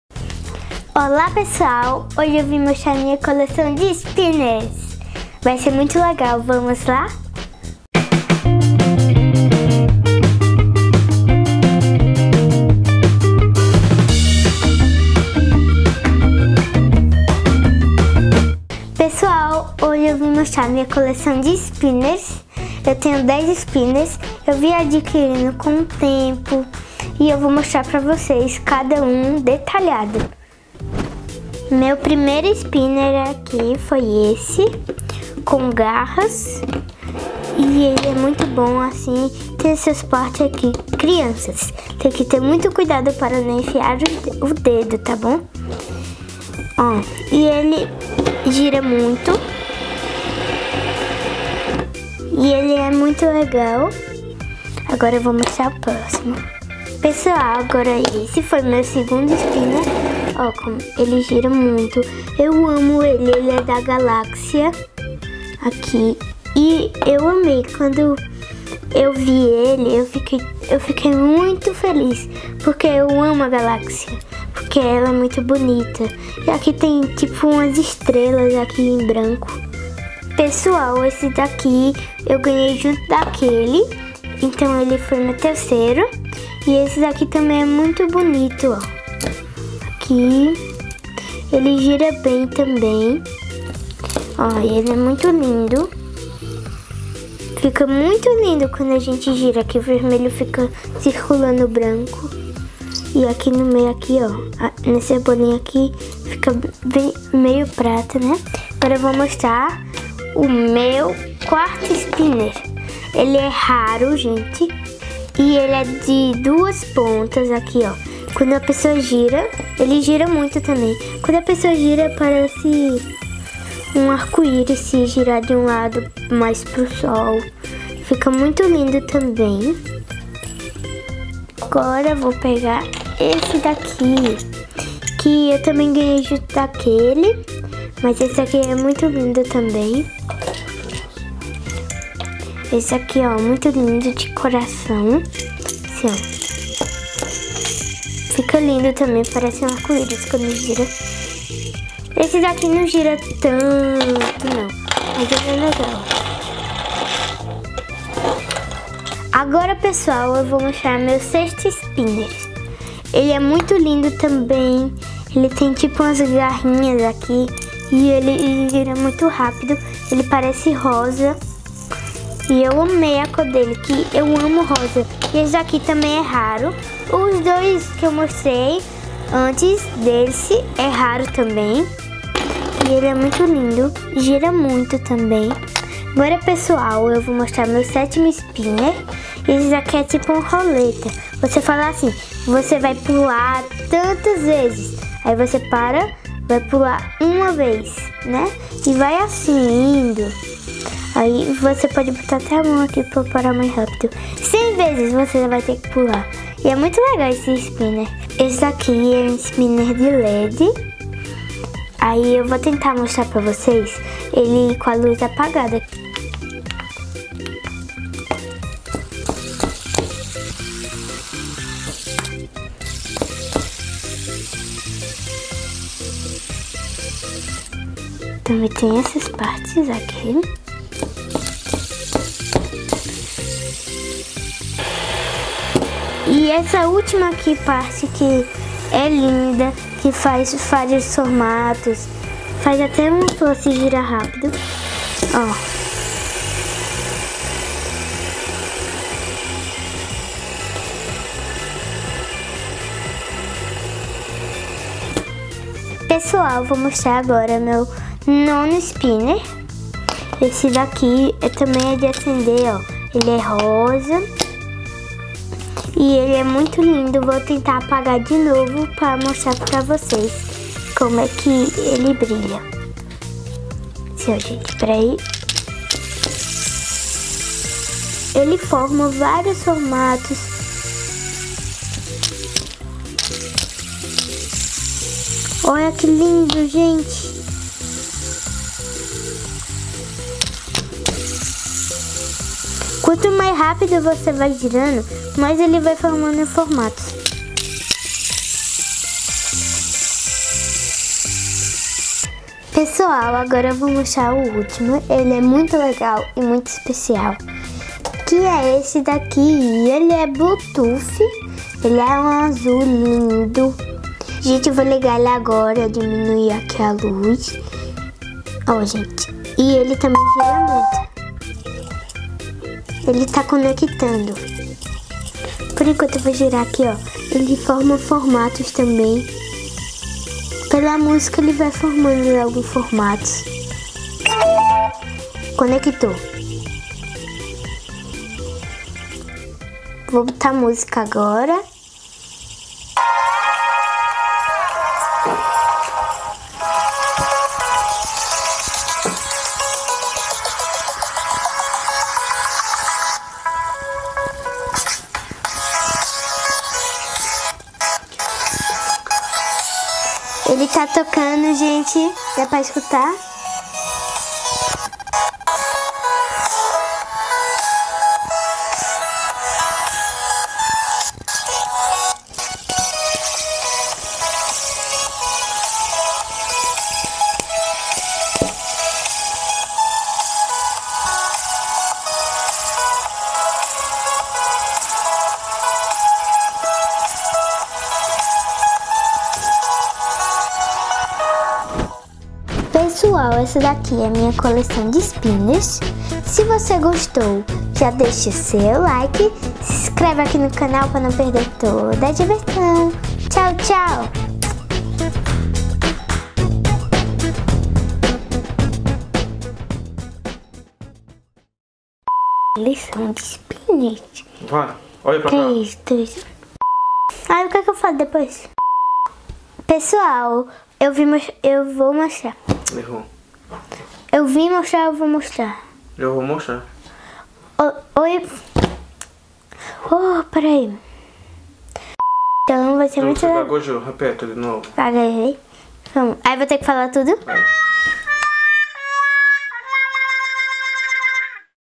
Coleção de Fidget Spinner
Oi pessoal! Hoje eu vou apresentar minha coleção de Fidget Spinners para vocês, alguns são raros, outros "diferentões", mas todos são muito legais!!!

Beijinhos!!! ❤❤❤